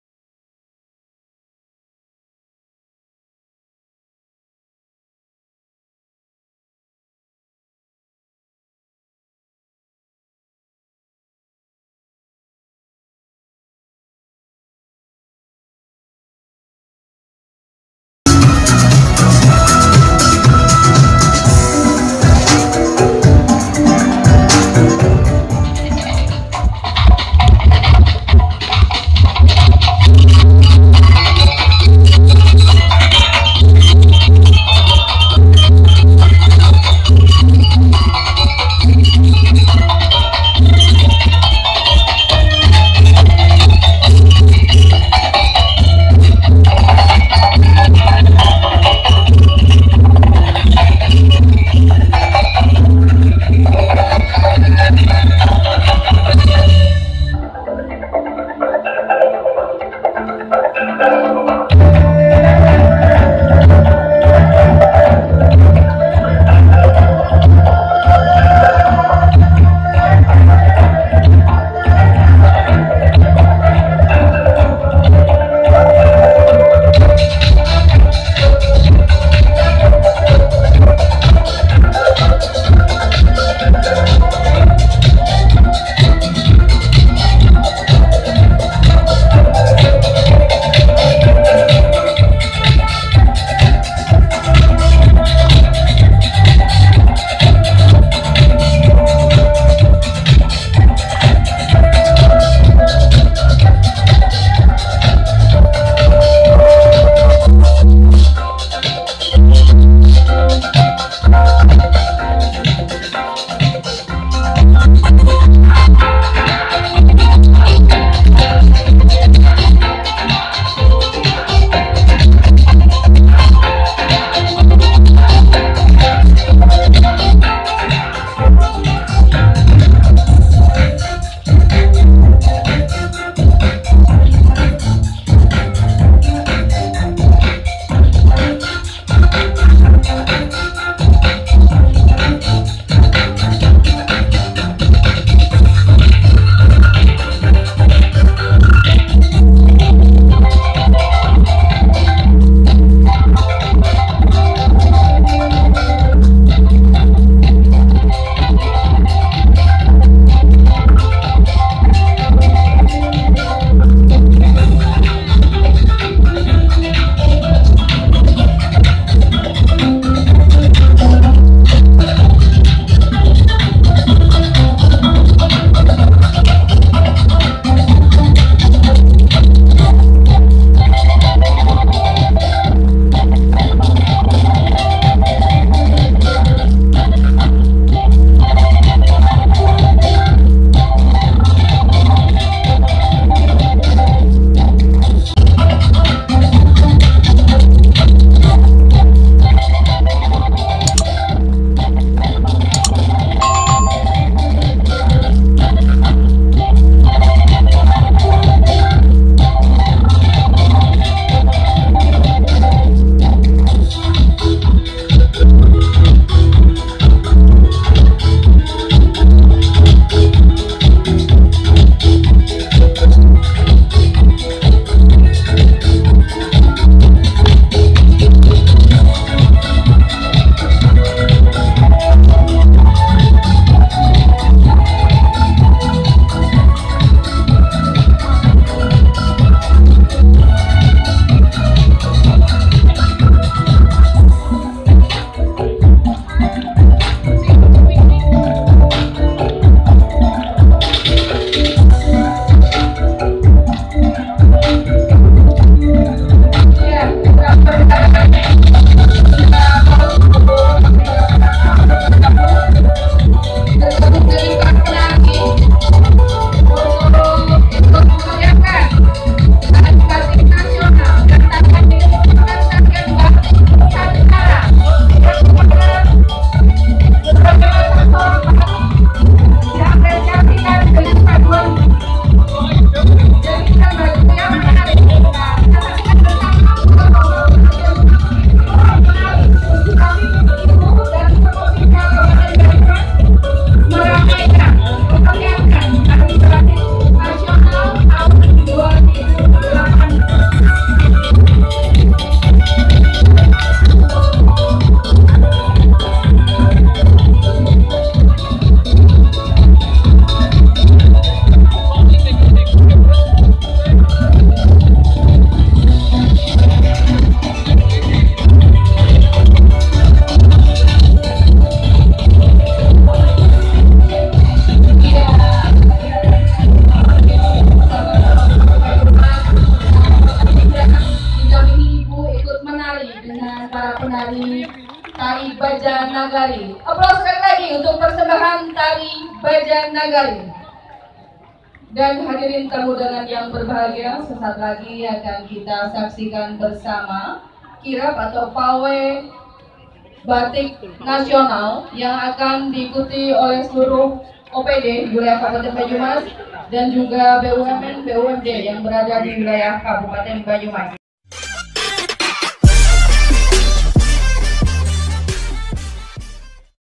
KANGEN AUFAA FEDORA LALITA TAMPIL UNTUK BANYUMAS PART 1
Kolaborasi Penari Bajanagari dan All Model dari Batik Anto Djamil dalam rangka Hari Batik Nasional Tingkat Kabupaten Banyumas di Alun-alun Purwokerto